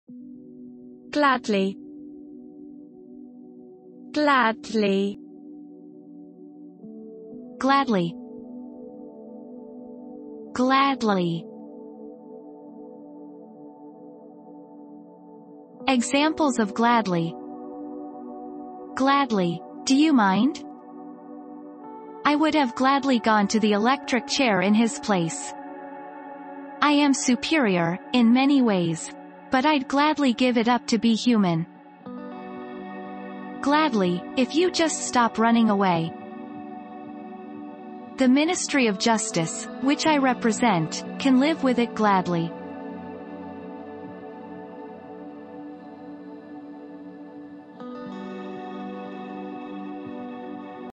GLADLY pronunciation • How to pronounce GLADLY
How to say Gladly in British English and American English?